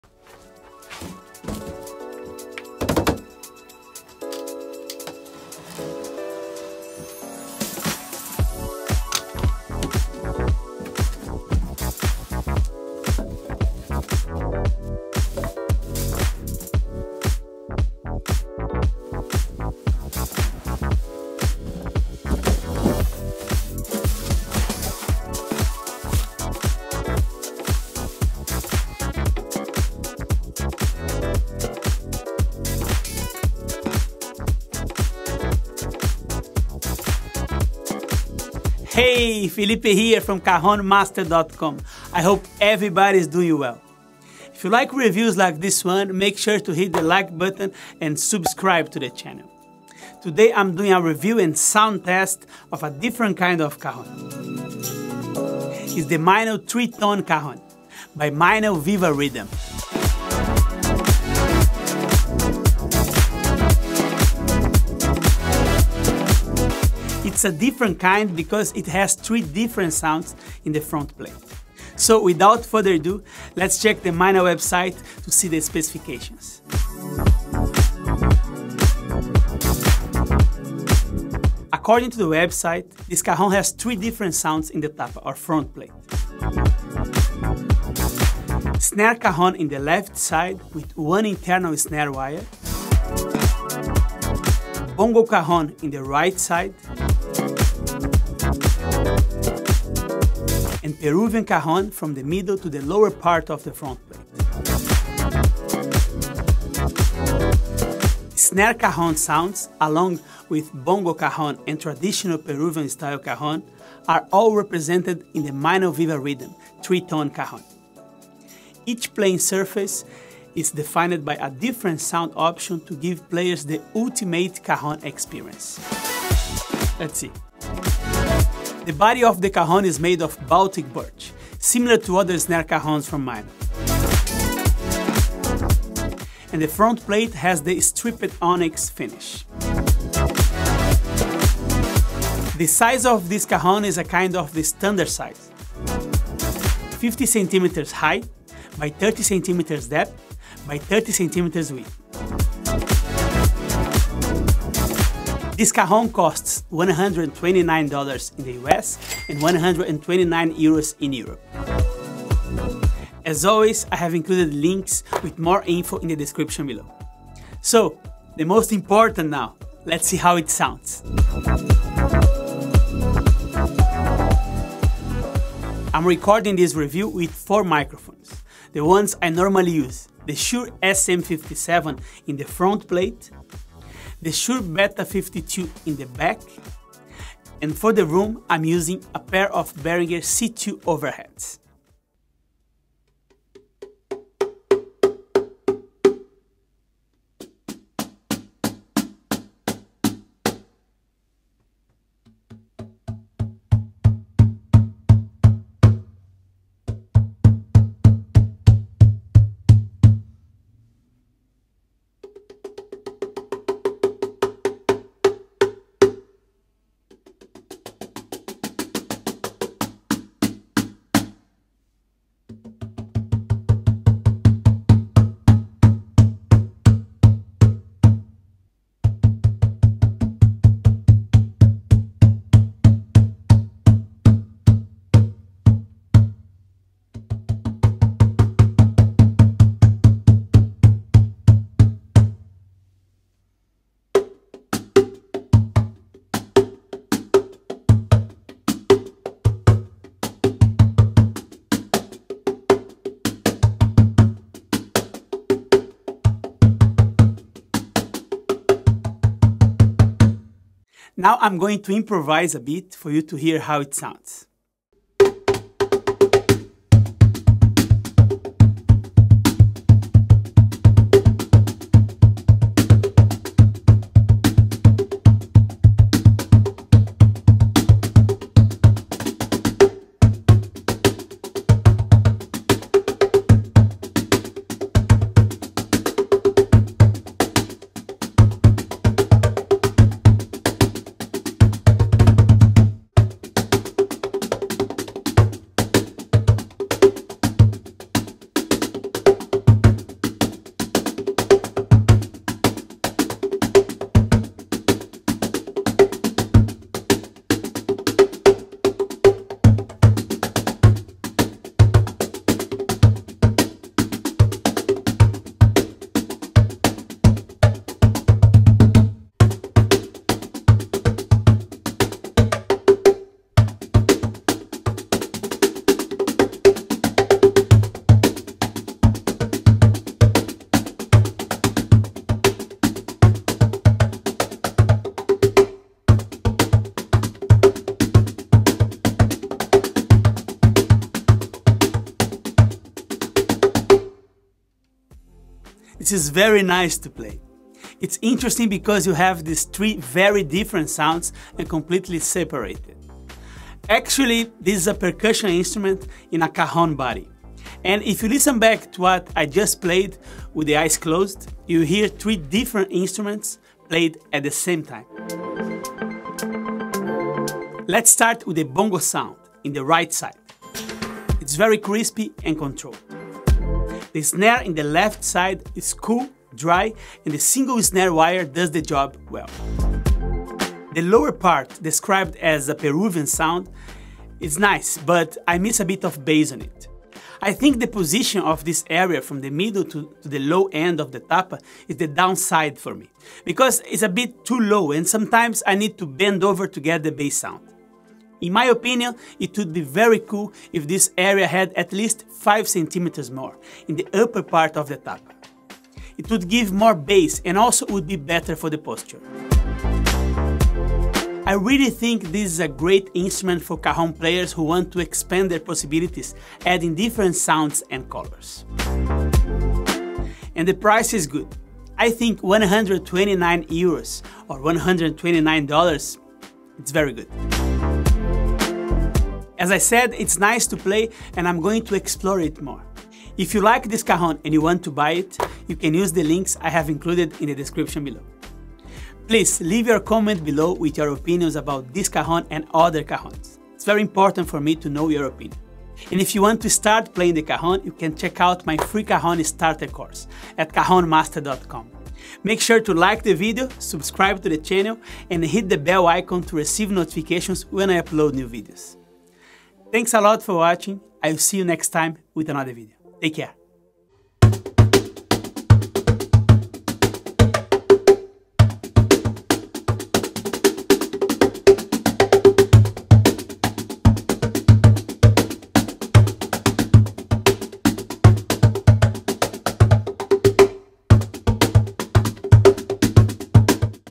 3in1 TRI TONE Cajon - Unbox Play-test & Review
3in1 TRI TONE Cajon - Unbox Play-test & Review
Attending requests, today I’m reviewing the Meinl VivaRhythm TRI TONE Cajon - VR-TRICAJ-SO.
This can be a great option if you are searching for different sounds in a cajon body.

To buy this Cajon, please use the links below, thanks!

☞ Mics I use:
Shure SM57

☞ Time stamps
0:03 Unboxing
1:20 Specs/Features
2:54 Sound Test
5:45 Review